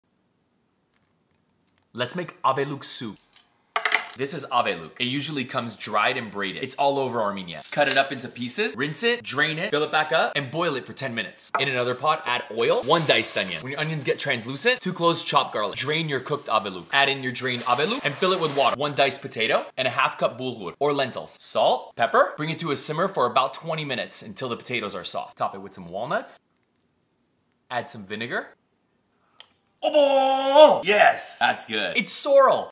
Aveluk Soup Armenian Sorrel Soup
Aveluk is sorrel that’s dried and braided all over Armenia. It’s usually made into a soup or a salad! It’s delicious and extremely healthy!